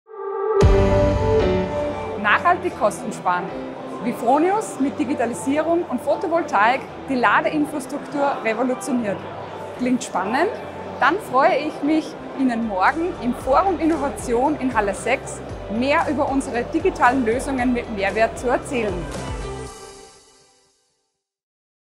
Fronius LogiMAT 2023 - Statement Lisa Fuchs (EN/DE)
Fronius revolutionizes sustainable charging with smart digital solutions and connecting photovoltaics to your power supply.

Fronius revolutioniert das nachhaltige Laden mit intelligenten digitalen Lösungen und der Integration von Photovoltaik in den Energiekreislauf. Kontaktieren Sie uns jetzt für mehr Informationen für eine optimale Batterieladestation.